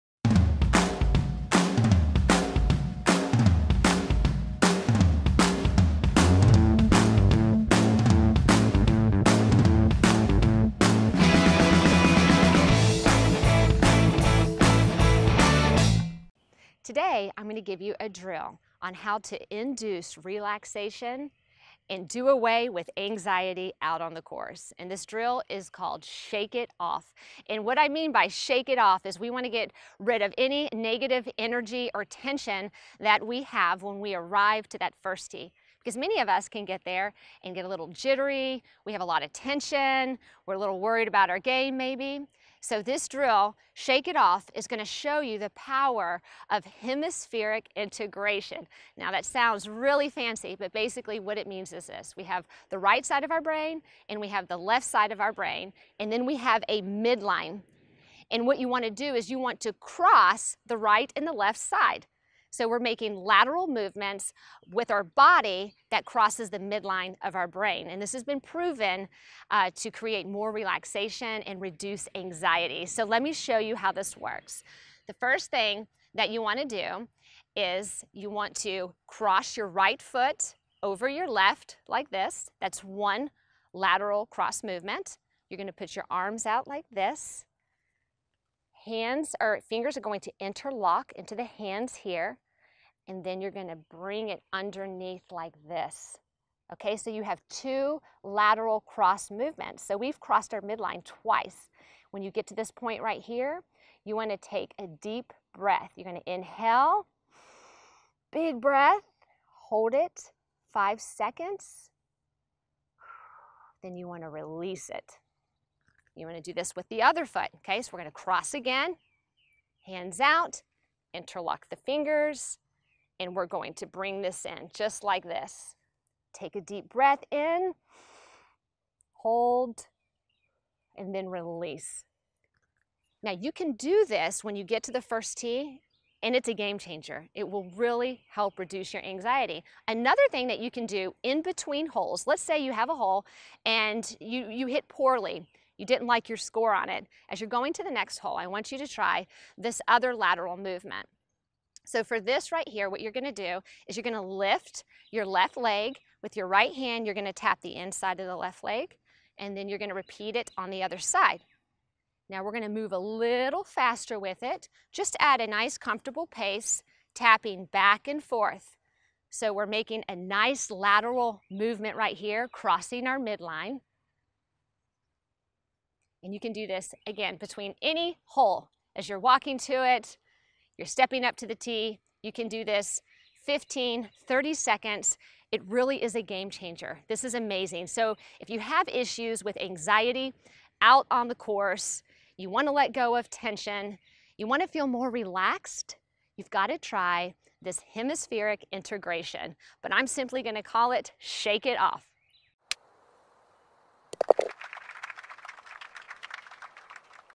Skill Drills with Meredith Kirk~Shake It Off
Meredith shares a drill that will help induce relaxation and do away with anxiety on the golf course.